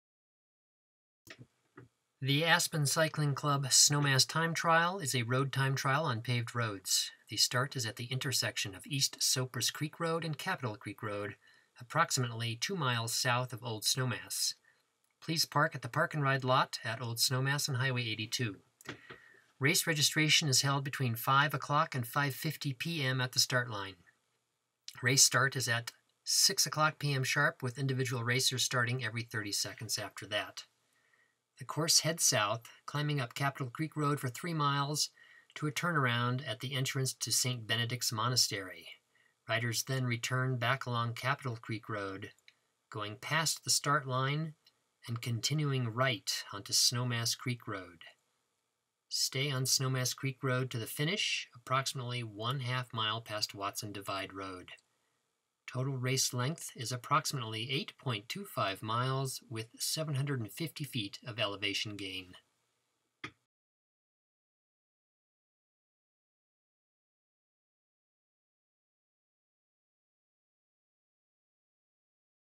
Aspen Cycling Club Snowmass Time Trial Course Review
The Aspen Cycling Club Snowmass Time Trial is a road time trial on paved roads. The start is at the intersection of East Sopris Creek Road and Capitol Creek Road, approximately 2 miles south of Old Snowmass. Please park at the park-n-ride lot at Old Snowmass on Highway 82. Race registration is held between 5:00 and 5:50 pm at the start line. Race start is at 6:00 PM sharp, with individual racers starting  every 30 seconds after that. The course heads south, climbing up Capitol Creek Road for 3 miles to a turnaround at the entrance to St. Benedict's Monastery. Riders than return back along Capitol Creek Rd, going past the start line, and continuing right onto Snowmass Creek Road. Stay on Snowmass Creek Road to the finish, approximately one half mile past Watson Divide Rd. Total race length is approximately 8.25 miles with 750 feet of elevation gain.